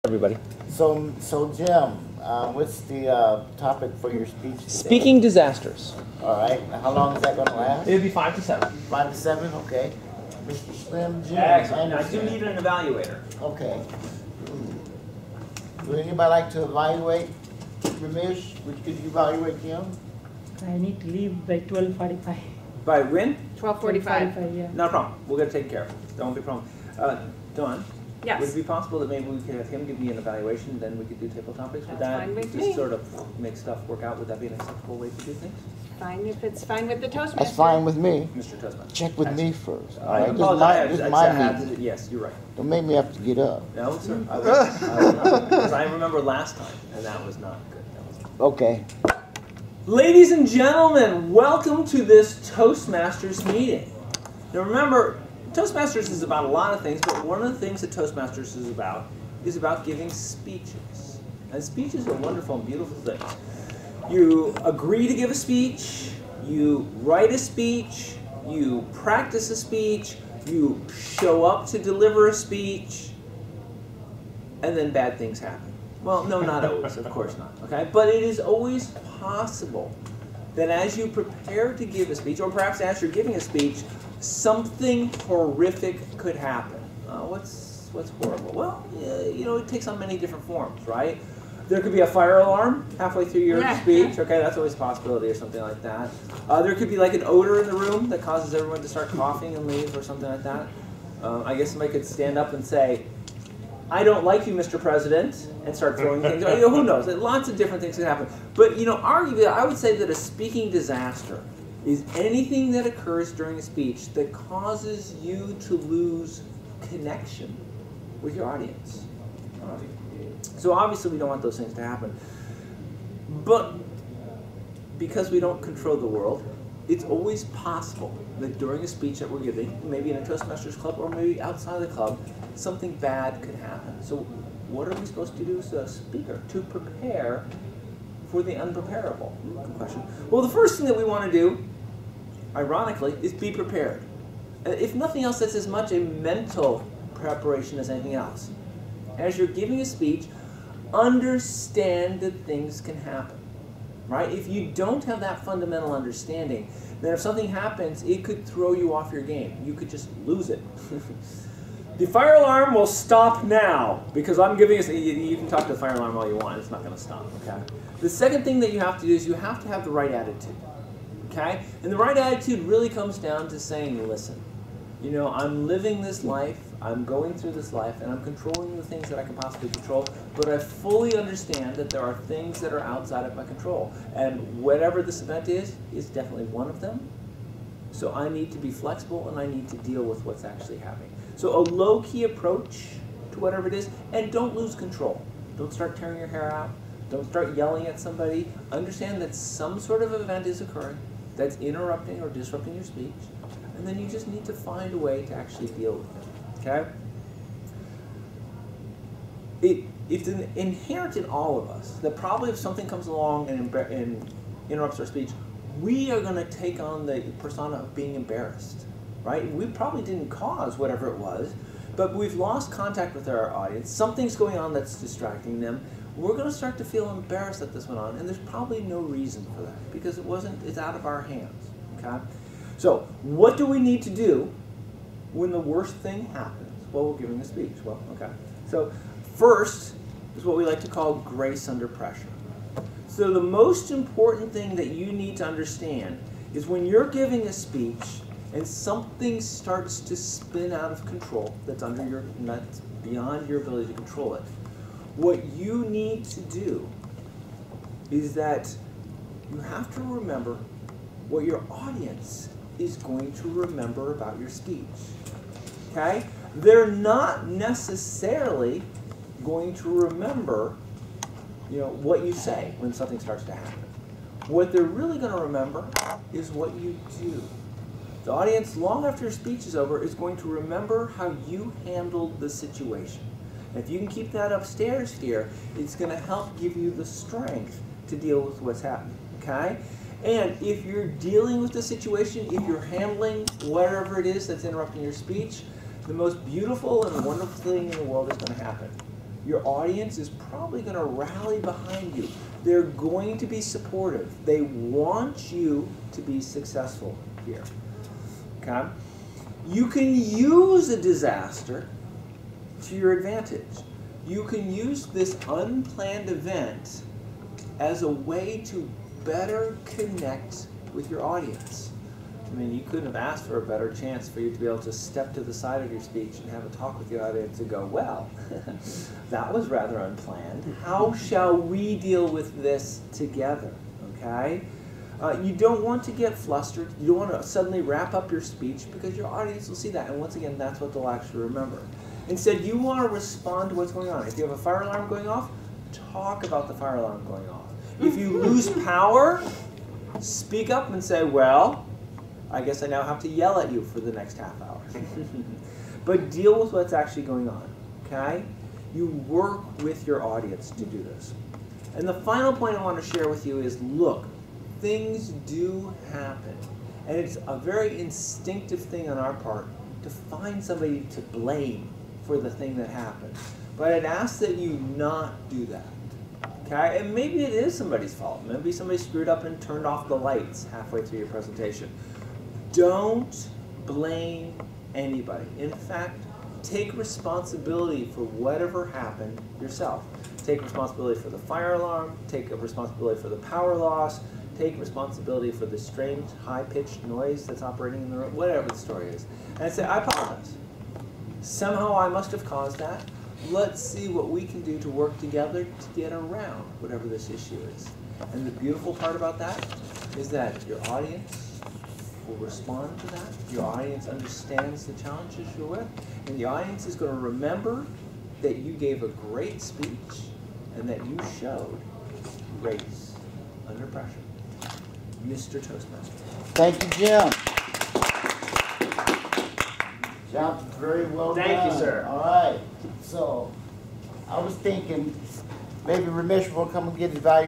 170316 Jim Speech
Jim gives a speech on how we can deal with speaking disasters when they occur.